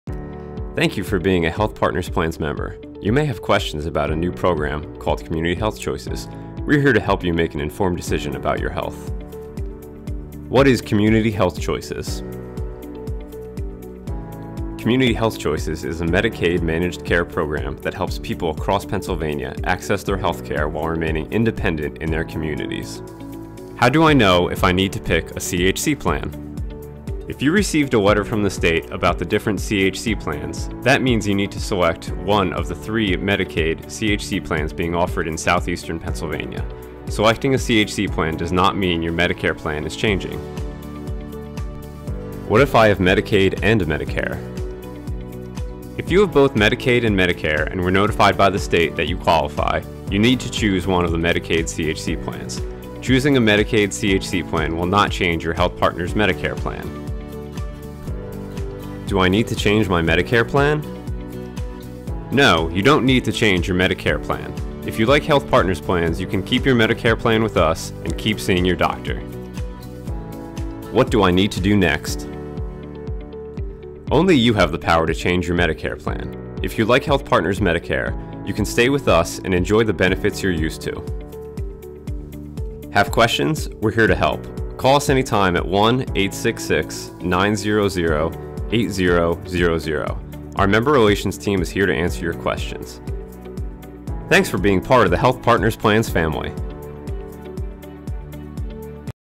Community Health Choices
Community Health Choices, or CHC, is a Medicaid managed care program that helps people across Pennsylvania access their health care while remaining independent in their communities.